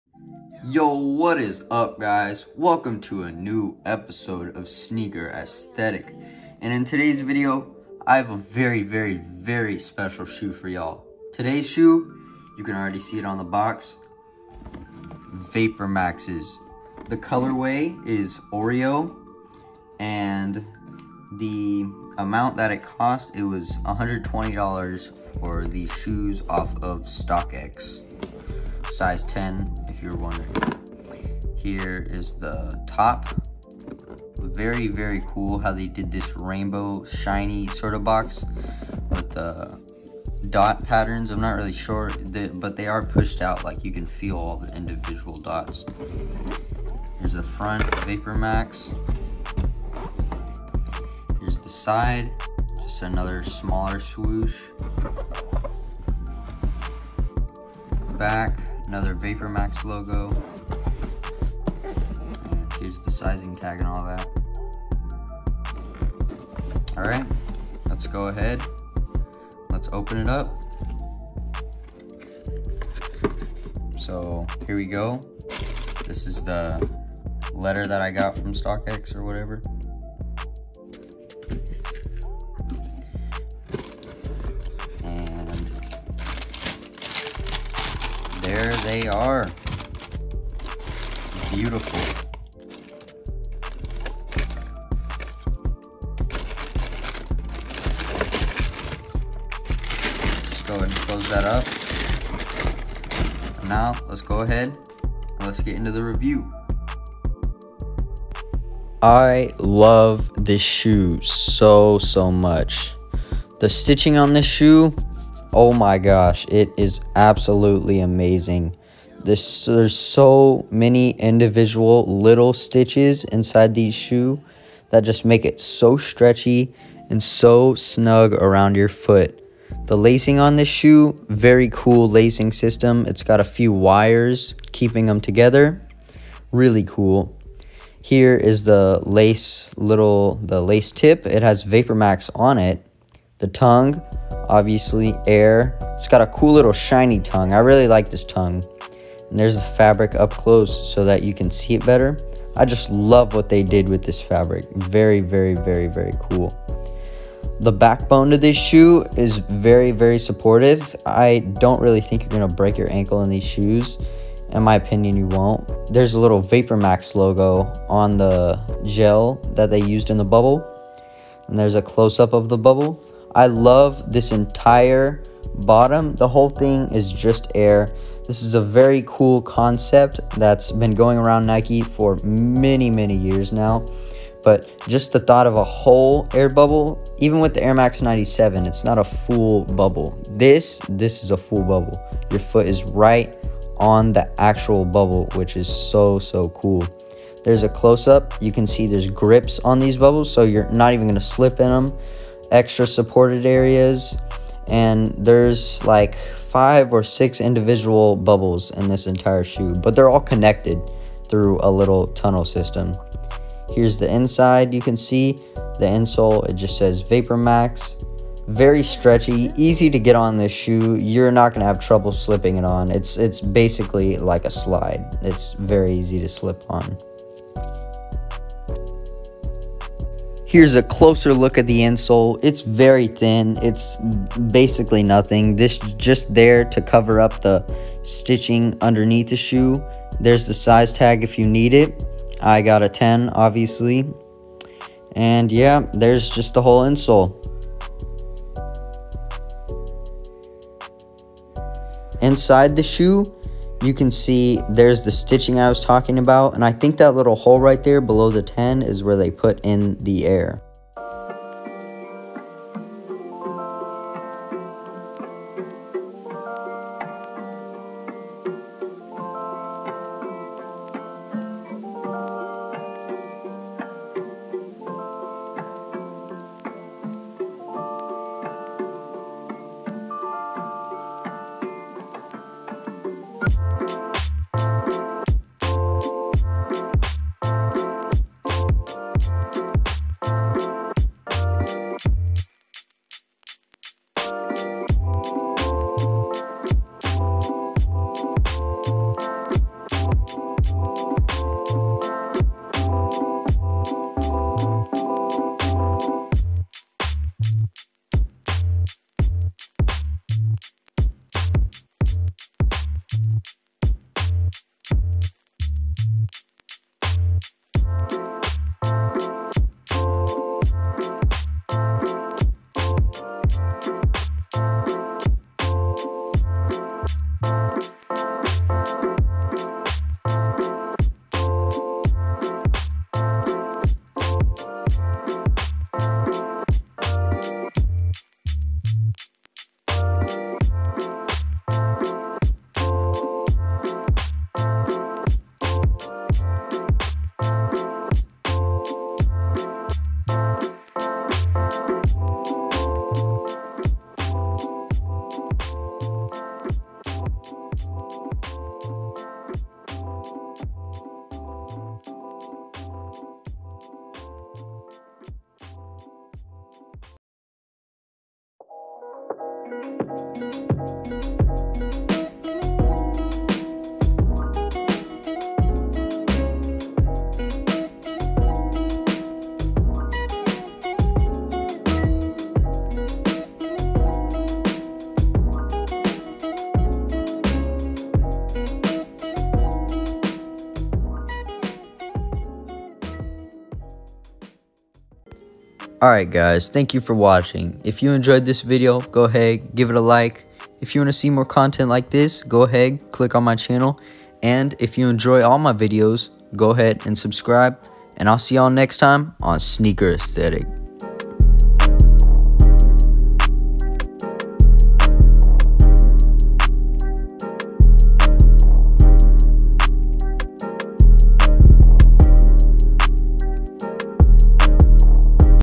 Oreo vapor max Nike
(Socials)

(Background music)

(Hashtags)

Unboxing, Review, Onfoot look at the Oreo Vapormax Nikes!!!